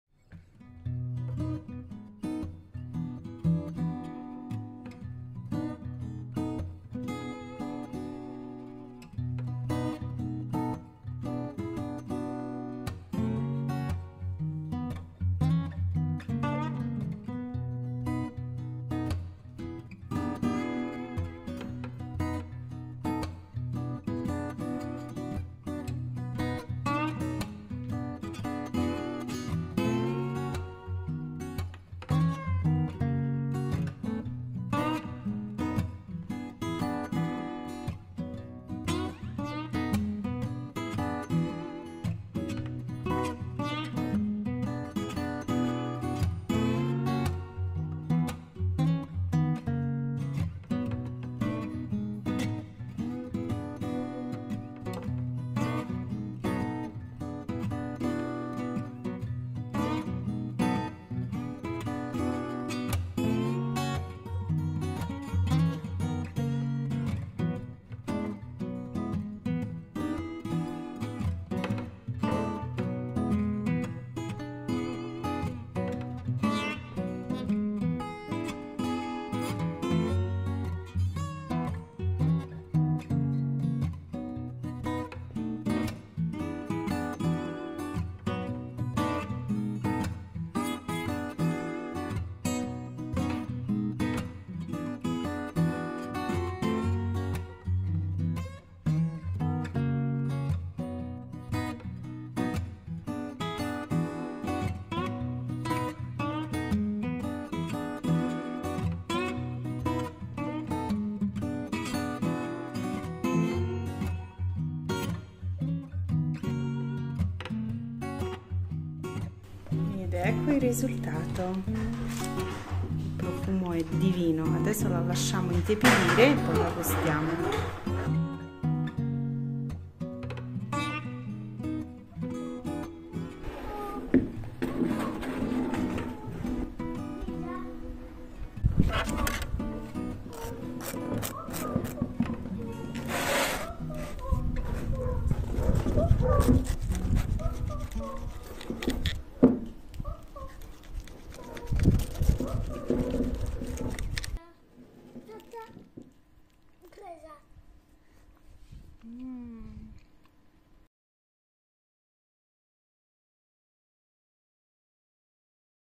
Torta salata di pasta sfoglia con pomodoro e mozzarella ROSA DI POMODORI di Ricette di Gabri
😋🍅Un rotolo di pasta sfoglia, pomodori freschi e un poco di formaggio...
Basta poco per fare una deliziosa torta salata estiva!
INGREDIENTI👇
1 rotolo di pasta sfoglia rotonda
3 pomodori grandi
2 manciate di parmigiano
mozzarella sgocciolata
basilico fresco
sale
olio

Se vi fa piacere, seguitemi anche su Facebook e Instagram 😉

▼ LISTA AMAZON ▼